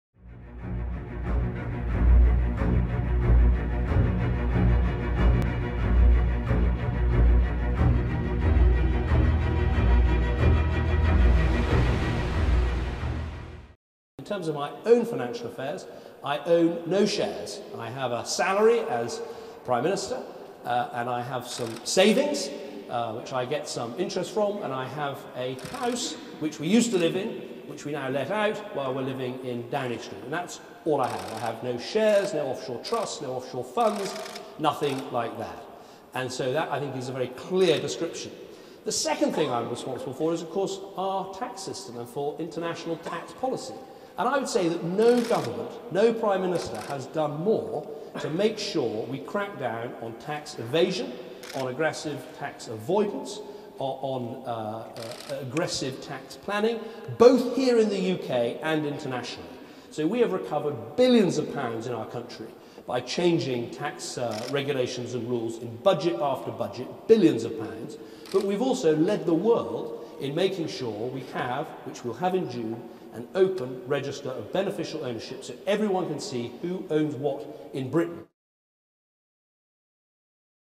Panama Papers: Cameron Response/Denial Video
UK PM David Cameron attempts to quell the growing PPap storm.